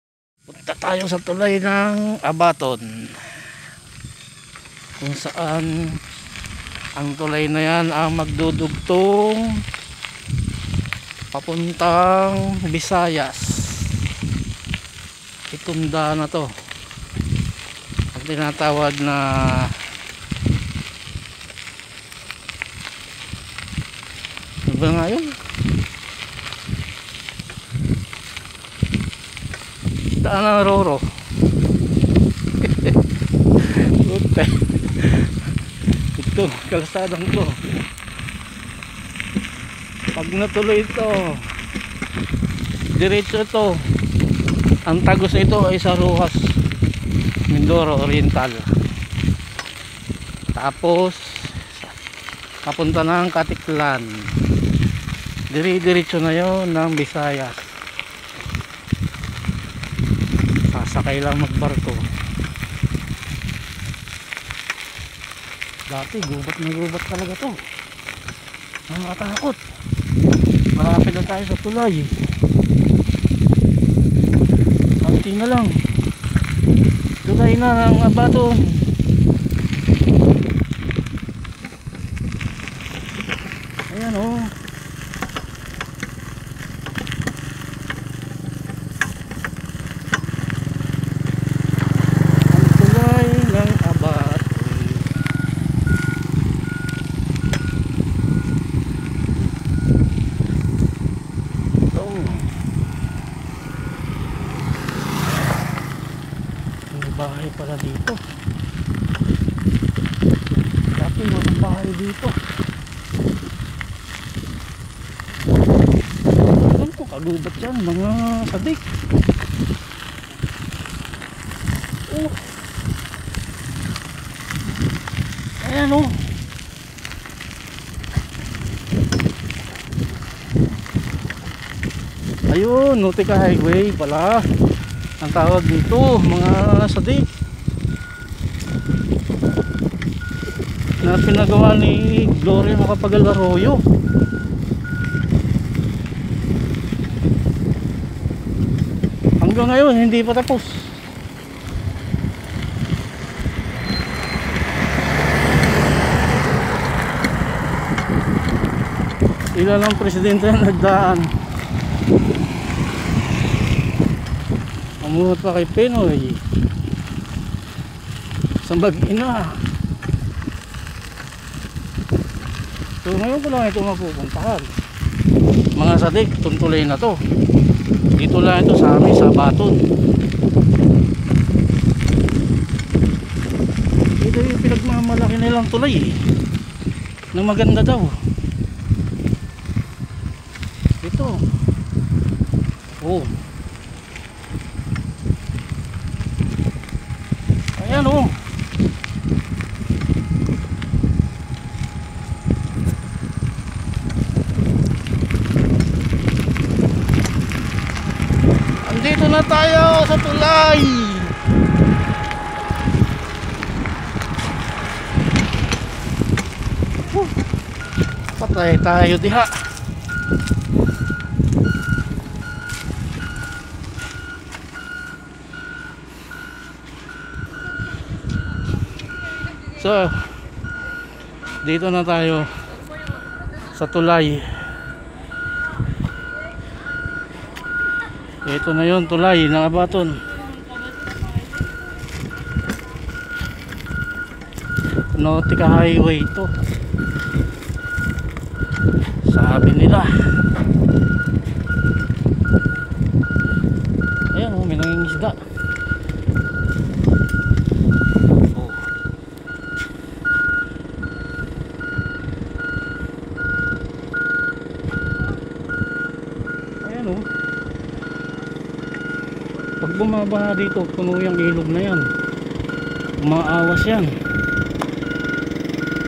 Ang tulay na magdudugtong papunta ng visayas.notica high way
ang tulay na ito nagdudugtong papunta sa visayas at notica higj way